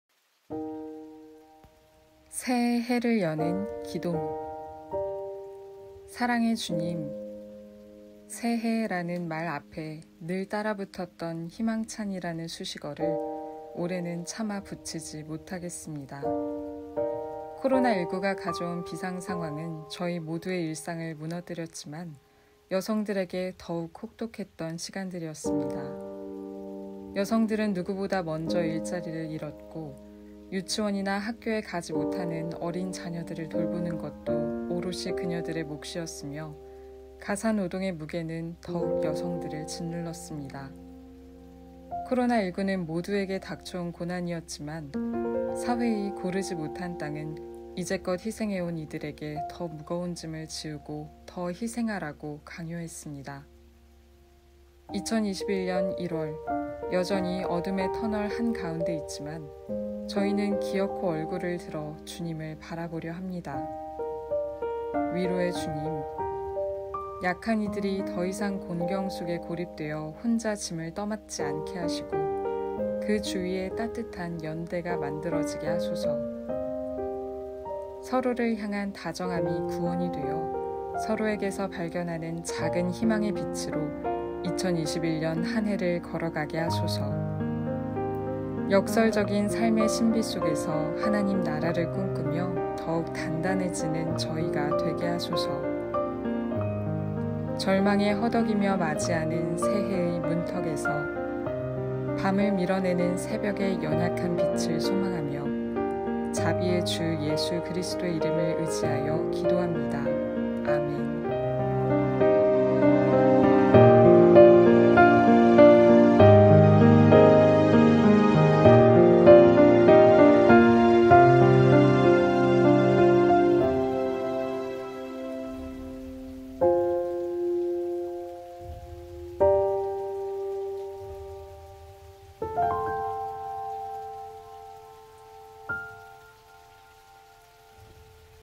서로를 향한 다정함이 구원이 되어(새해를 여는 기도)_이수연 목사
기독교반성폭력센터의
서포터즈 1기 "센기반"이 연재했던
기독교 페미니스트의 기도 시리즈,
[안나의 기도]가 움트다와 만나
영상으로 제작되었습니다.

마지막 기도는
“새해를 여는 기도”입니다.
가을을 맞이하고 있는 이 때에 다시금 새해의 시작을 생각하며
함께 기도해요.